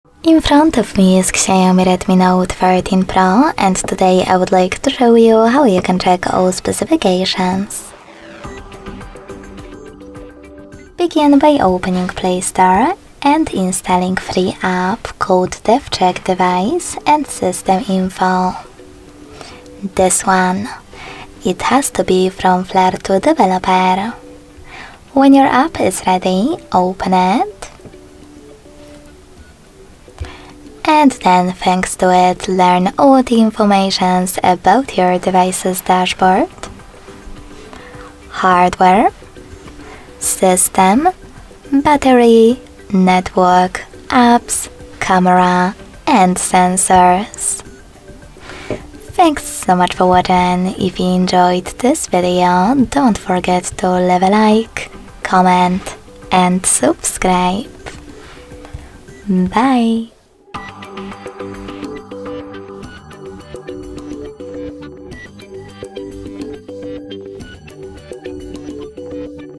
How to Check Phone Specification on XIAOMI Redmi Note 13 Pro? | DevCheck Hardware & System Info
Find out more:
Unlock the full potential of your XIAOMI Redmi Note 13 Pro by learning how to check its detailed specifications using the DevCheck Hardware & System Info app. Understanding your device's specifications is essential for optimizing performance, troubleshooting issues, and ensuring compatibility with apps and accessories. In this comprehensive tutorial, tailored specifically for the XIAOMI Redmi Note 13 Pro, you'll discover how to use the DevCheck app to access a comprehensive overview of your device's hardware and system information. From CPU and GPU details to battery health and sensor data, you'll gain valuable insights into the capabilities of your device.

Why is it important to check the phone specifications on the XIAOMI Redmi Note 13 Pro?
What types of specifications can I expect to find using the DevCheck Hardware & System Info app?
How can knowing my device's specifications help me optimize performance and troubleshoot issues?
Is the information provided by the DevCheck app accurate and reliable?
How do I download and install the DevCheck Hardware & System Info app on my XIAOMI Redmi Note 13 Pro?